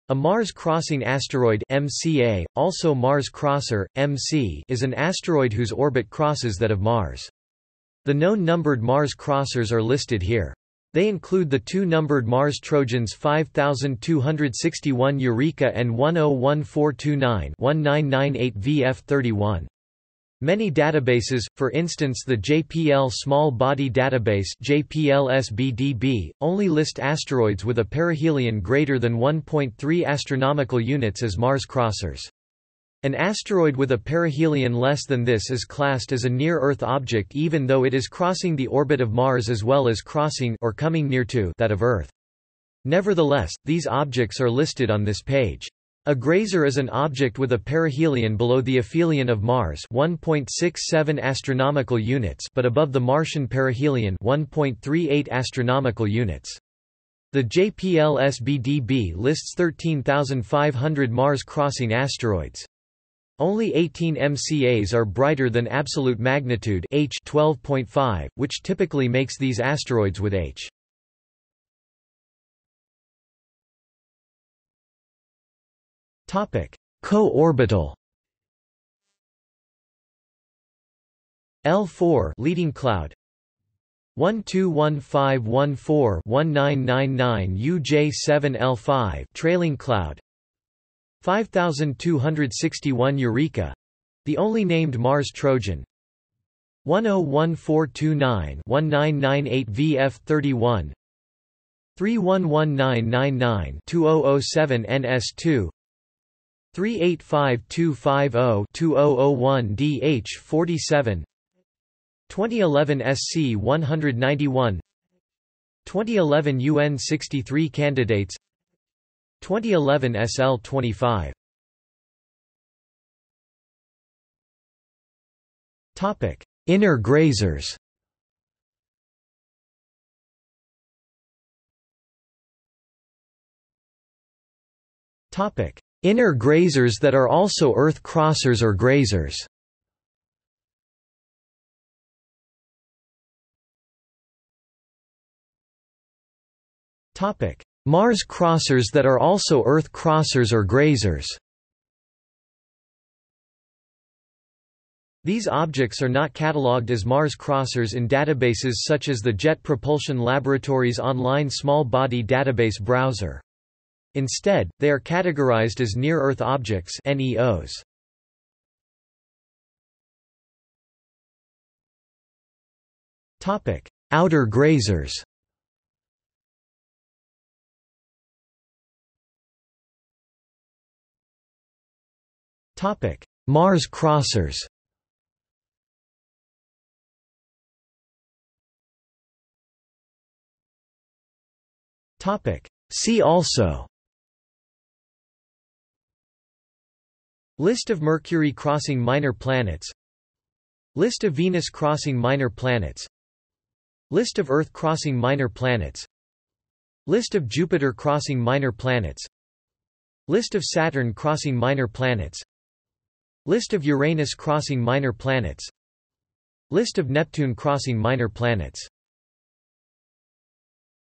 Mars-crosser | Wikipedia audio article
00:01:12 1 Co-orbital
00:02:02 2 Inner grazers
00:02:11 3 Inner grazers that are also Earth-crossers or grazers
00:02:23 4 Mars-crossers that are also Earth-crossers or grazers
00:02:49 5 Outer grazers
00:02:59 6 Mars-crossers
00:03:08 7 See also

- Socrates

SUMMARY
A Mars-crossing asteroid (MCA, also Mars-crosser, MC) is an asteroid whose orbit crosses that of Mars. The known numbered Mars-crossers are listed here. They include the two numbered Mars trojans 5261 Eureka and (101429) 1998 VF31.
Many databases, for instance the JPL Small-Body Database (JPL SBDB), only list asteroids with a perihelion greater than 1.3 AU as Mars-crossers. An asteroid with a perihelion less than this is classed as a near-Earth object even though it is crossing the orbit of Mars as well as crossing (or coming near to) that of Earth. Nevertheless, these objects are listed on this page. A grazer is an object with a perihelion below the aphelion of Mars (1.67 AU) but above the Martian perihelion (1.38 AU). The JPL SBDB lists 13,500 Mars-crossing asteroids. Only 18 MCAs are brighter than absolute magnitude (H) 12.5, which typically makes these asteroids with H12.5 more than 13 km in diameter depending on the albedo. The smallest known MCAs have an absolute magnitude (H) of around 24 and are typically less than 100 meters in diameter.